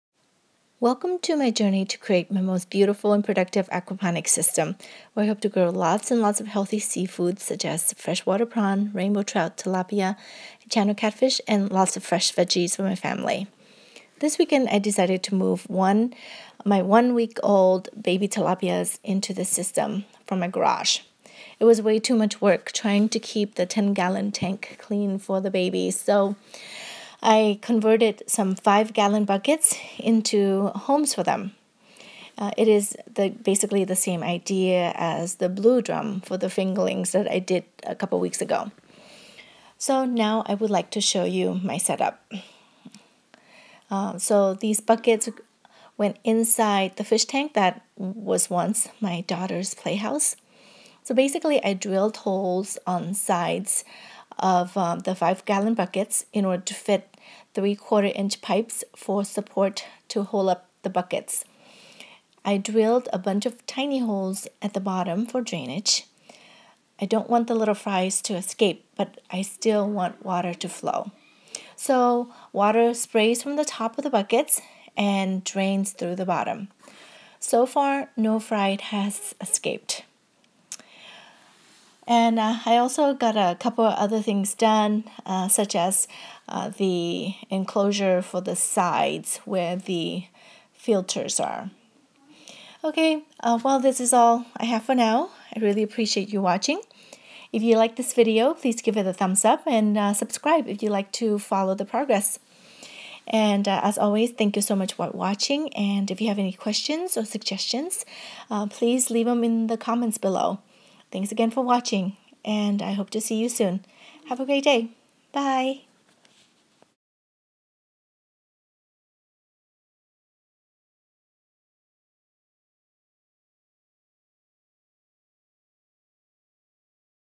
Most Beautiful and productive Aquaponics system S1 Ep13 bucket for fry and duckweed 1
Fresh water prawn, tilapia, trout, channel catfish and veggies can be grown in an Aquaponics System.  I’m working towards making my System beautiful and productive.

In this episode I showed my setup for one week old tilapia fry and how I grow duckweed in my System.